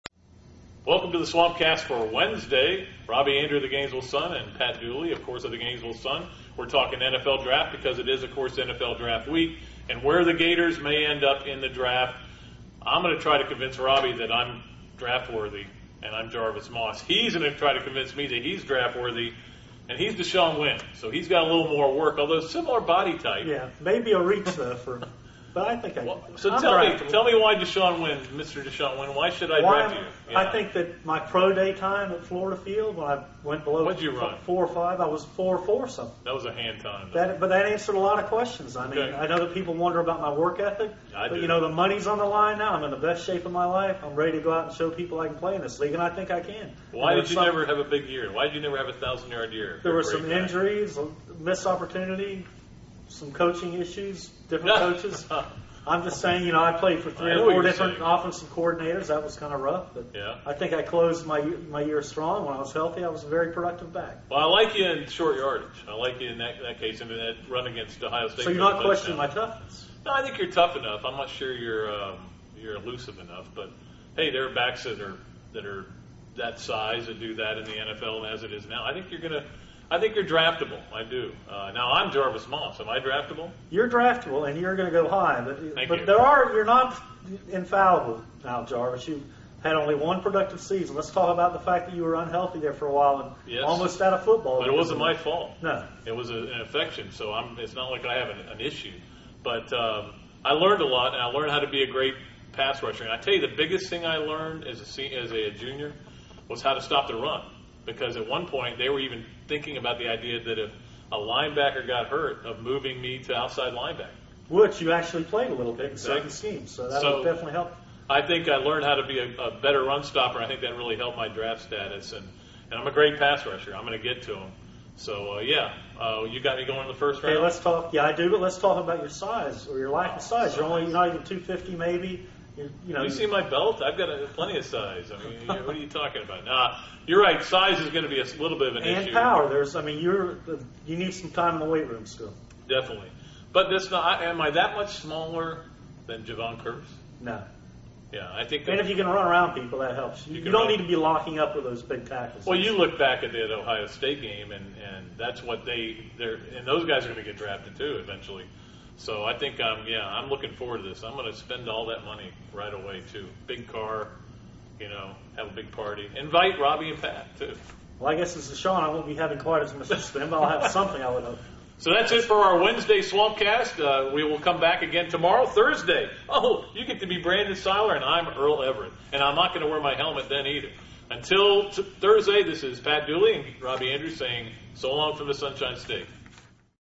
NFL Draft Wednesday
Robbie Andreu and Pat Dooley discuss Jarvis Moss and DeShawn Wynn's chances of being drafted this year.